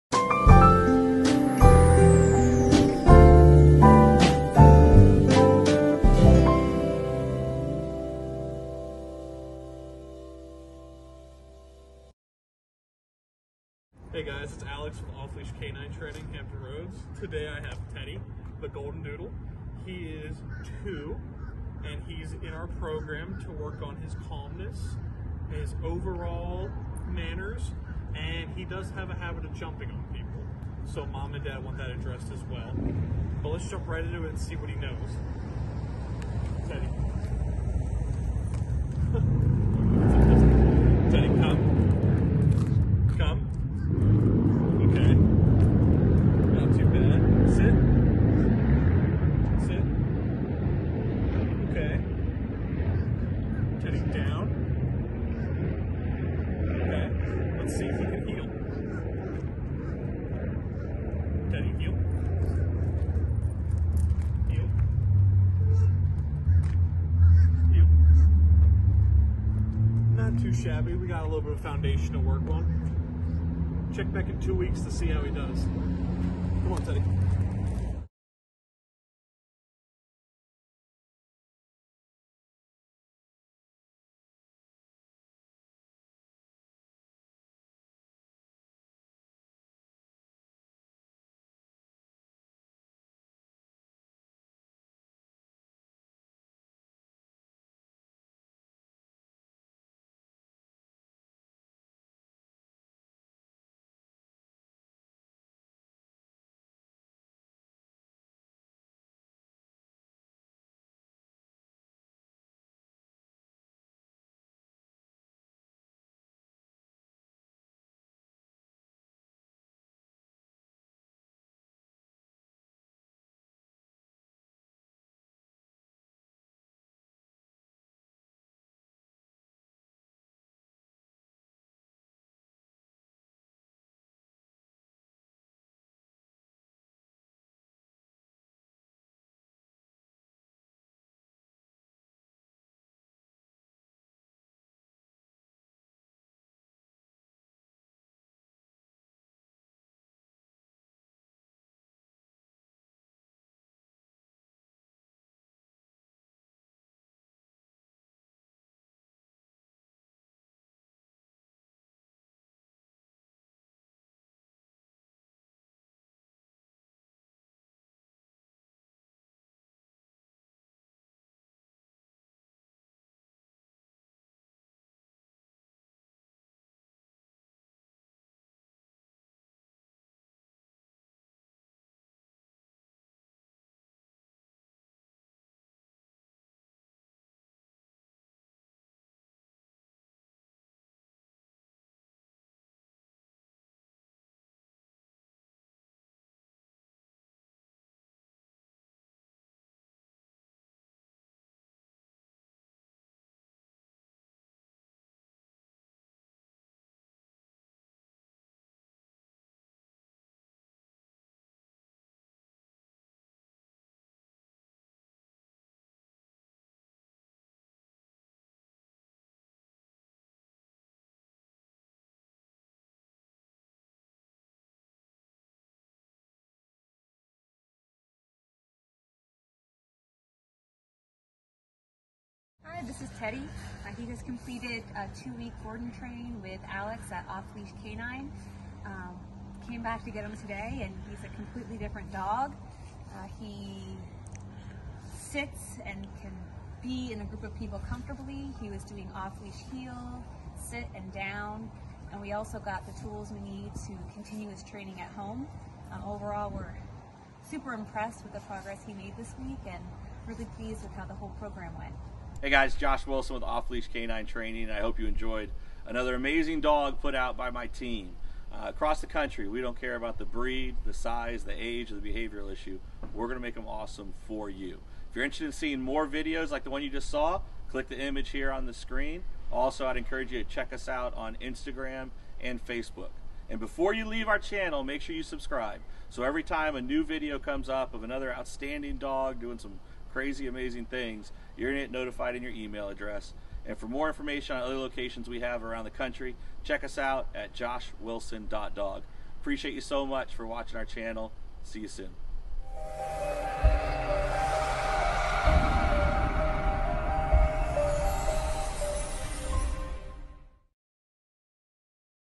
2 Y/O Golden Doodle "Teddy" | From stubborn to well behaved | Hampton Roads Dog Trainers
Witness the incredible transformation of Teddy, a 2-year-old Golden Doodle, as he transitions from a stubborn pup to a well-behaved and obedient companion with the help of the talented trainers at Hampton Roads Dog Trainers. This heartwarming video showcases Teddy's journey and highlights the power of effective training techniques.

🐾 Follow Teddy's remarkable progress as he learns to respond to commands with focus and enthusiasm. Through positive reinforcement and personalized training methods, the dedicated trainers at Hampton Roads Dog Trainers guide Teddy towards becoming a model of good behavior.

💪 Experience Teddy's journey as he overcomes his stubborn tendencies and develops into a well-mannered dog who eagerly follows instructions. From leash walking and basic commands to advanced obedience skills, Teddy's transformation is truly awe-inspiring.

🌟 Discover the dedication and expertise of the trainers as they patiently work with Teddy, providing him with the tools and guidance he needs to succeed. Through consistent training sessions and a nurturing environment, they help Teddy reach his full potential.